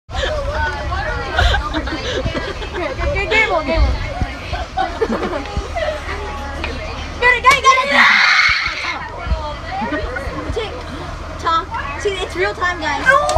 EPIC WATER BOTTLE CAP ON BUS!
Water Bottle cap on bus 3 bottles stacked!